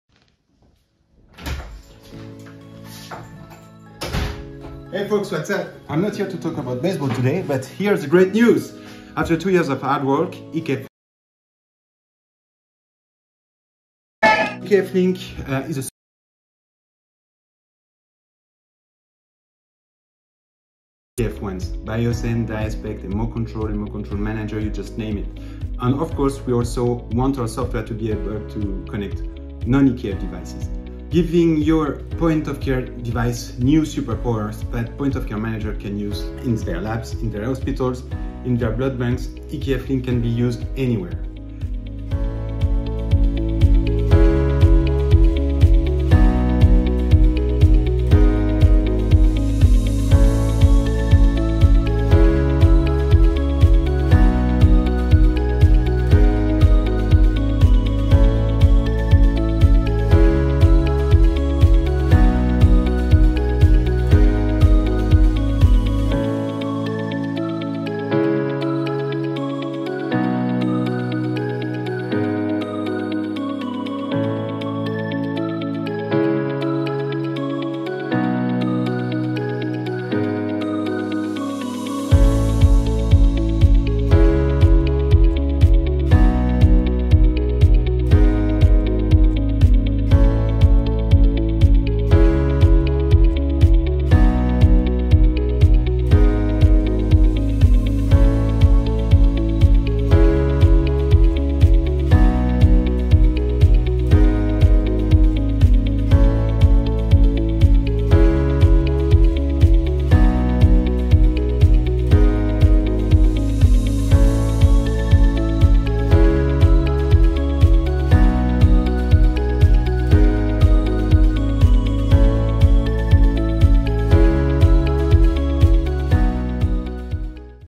Introducing EKF Link - Connectivity Solution
Connect all of your analyzers on one software platform.

EKF Link is a middleware solution for healthcare providers to manage all of their Point of Care analyzers and the generated data on one centralized platform.
EKF Link is a flexible, open solution that can be interfaced to any vendor’s Point of Care analyzer to permit the real-time management of data including patient test results, QC results, operator management and analyser configuration.
EKF Link enables the transfer of data from the analyzer to the hospital or laboratory database for subsequent processing.